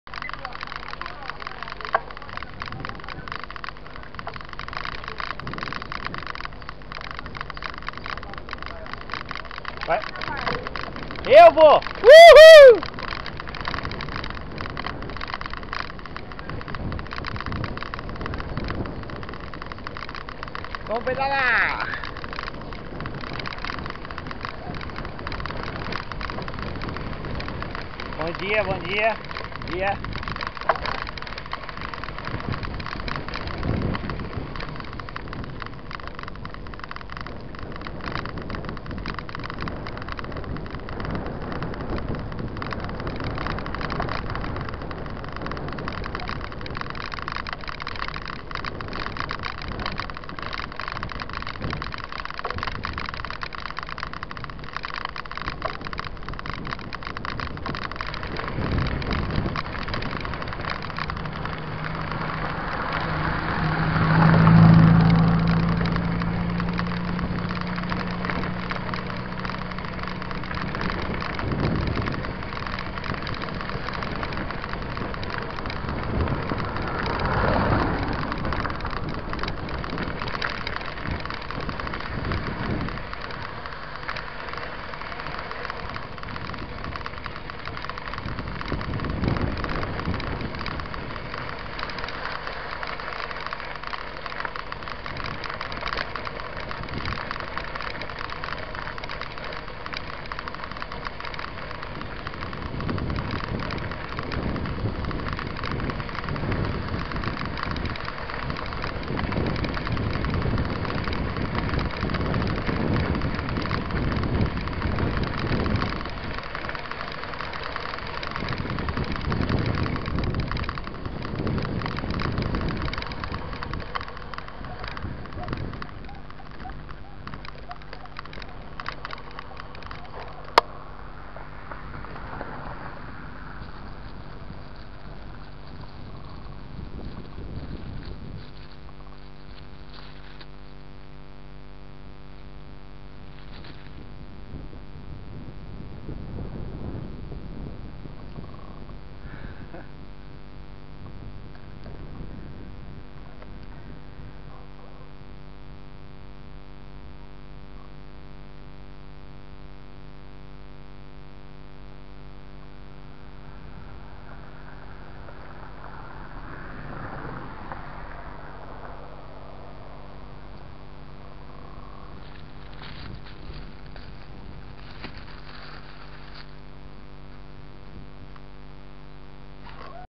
Mtb, Trilha, Cachoeira do Triângulo, 26 amigos, 49 km, Taubate, 2016, 36
Venha pedalar conosco, vamos pedalar,  vamos conhecer as cachoeiras da Serra da Mantiqueira.
4k, Ultra HD, Full HD, Mountain bike, Taubaté, SP, Brasil, 10 de Setembro de 2016.